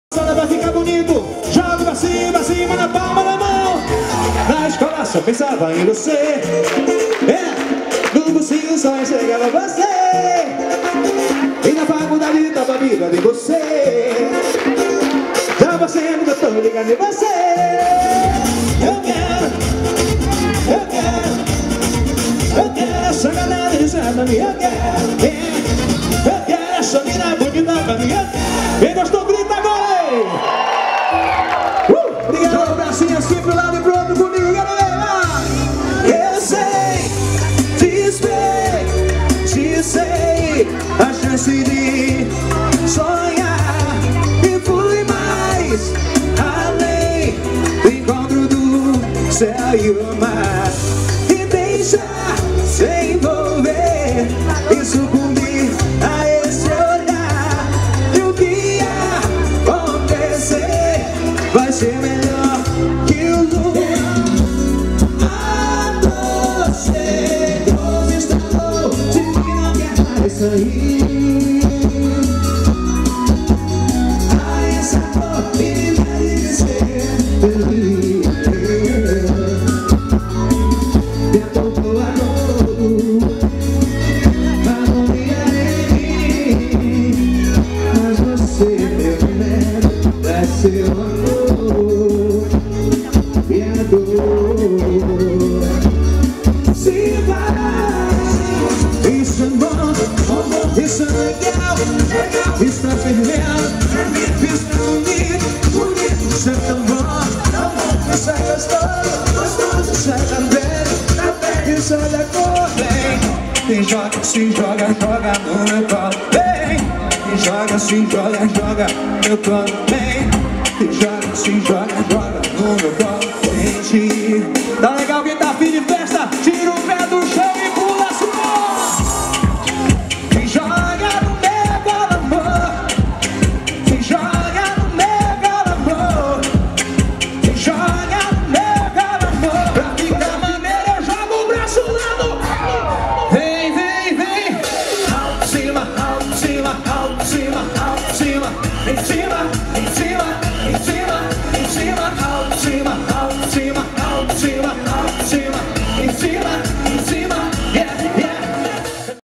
Jorginho Gomez - Aniversário de Belém na Praça Batista Campos
Melhores momentos do show realizado na Praça Batista Campos em 12 de janeiro de 2014, na programação do Sesi/Fiepa em parceria com a Tv Liberal.